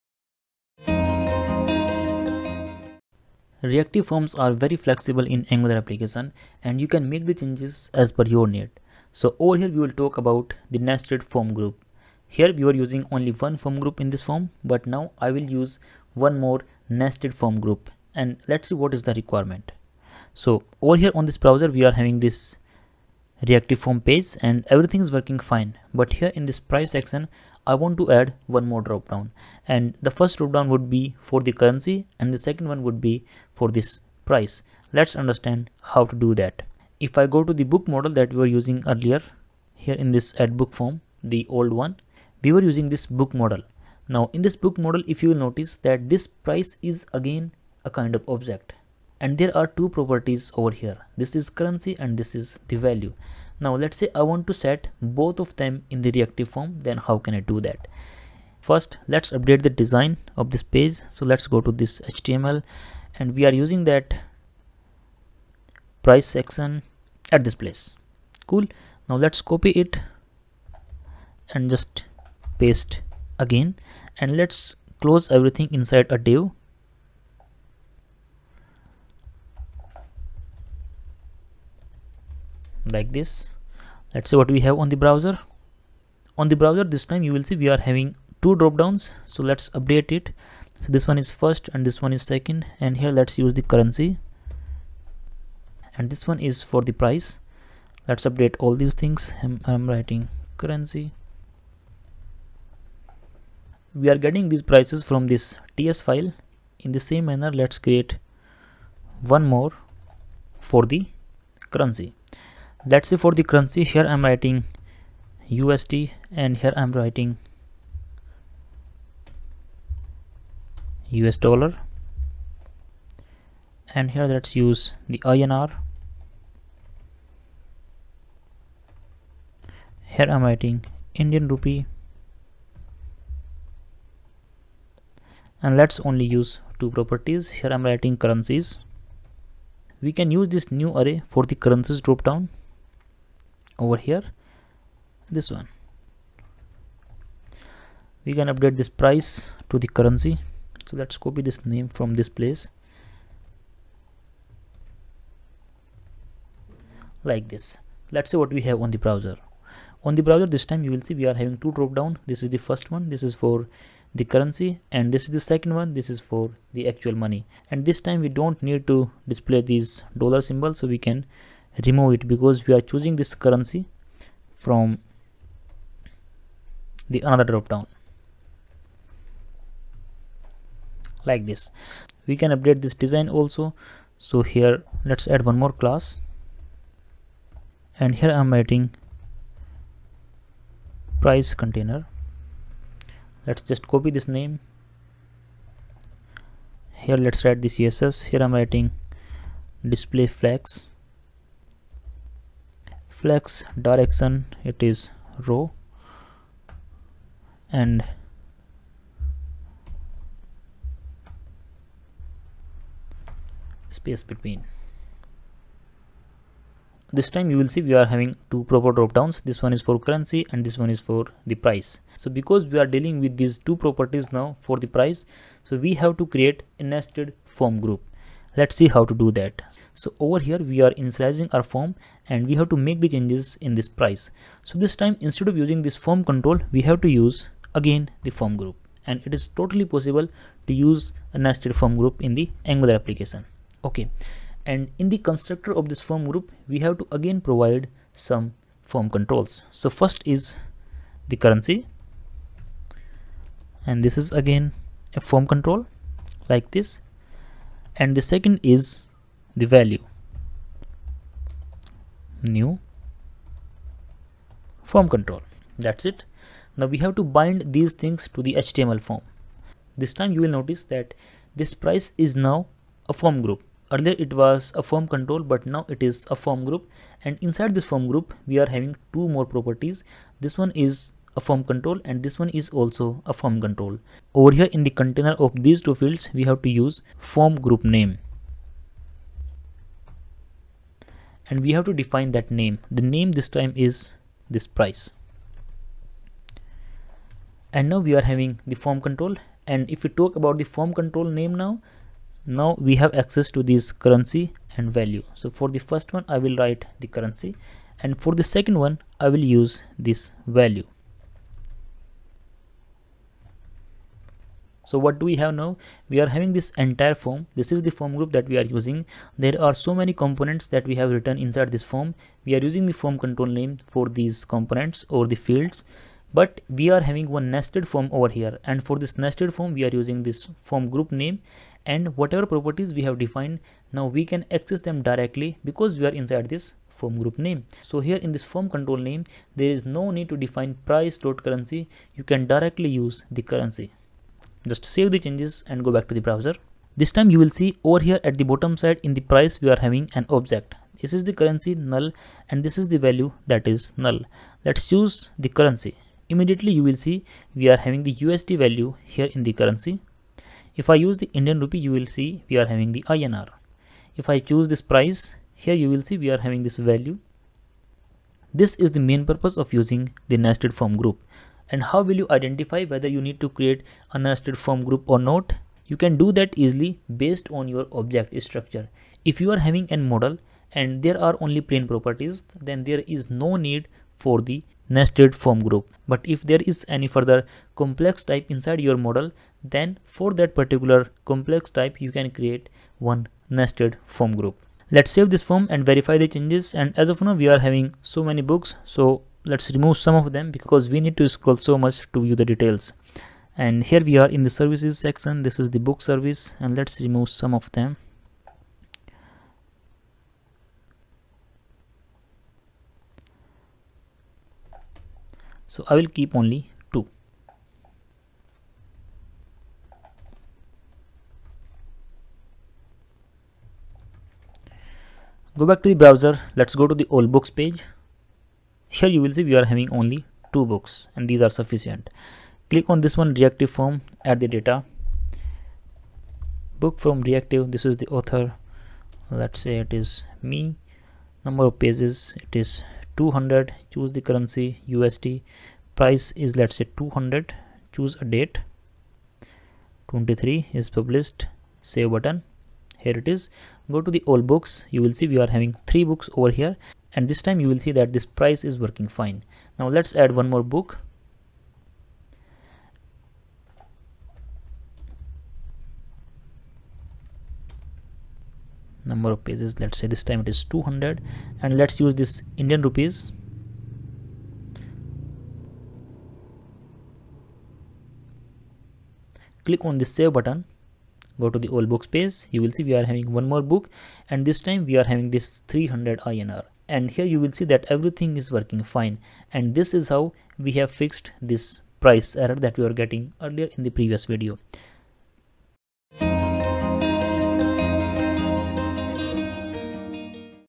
Nested Form Group Angular Reactive Forms | Reactive Forms in Angular | Complete Angular Tutorial
Angular Tutorial: Here you will learn about the Nested Form Group Angular Reactive Forms. In Angular Reactive Forms, you can create nested form groups by defining a FormGroup as a property of another FormGroup. This allows you to manage complex form input structures that have multiple levels of nesting.
To create the nested FormGroup, we use the formGroupName directive to indicate that the child form controls belong to the "address" group. We then define the child form controls inside a div element that has the formGroupName directive.

In the constructor of the component, we create the FormGroup hierarchy using the FormBuilder. We define the "address" control as a nested FormGroup and set its initial values to empty strings.

Finally, we define a submit() method that logs the form values to the console when the "Submit" button is clicked.